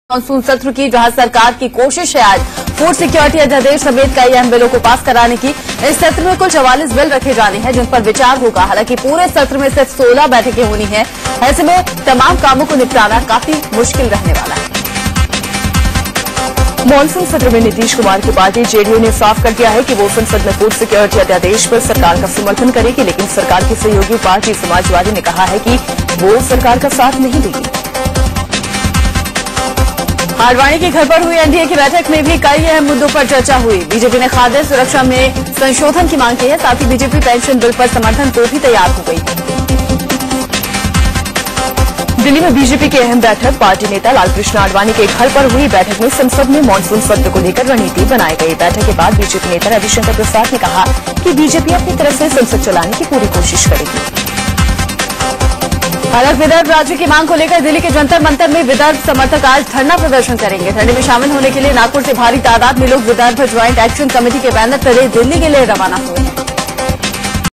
India News : Monsoon session of Parliament begins today
India News : A heavy agenda including the ordinance on the Food Security Bill awaits the Monsoon session of Parliament beginning on Monday amid expectations that the short sitting will be more businesslike and smooth as compared to the din and dust in the last few sessions.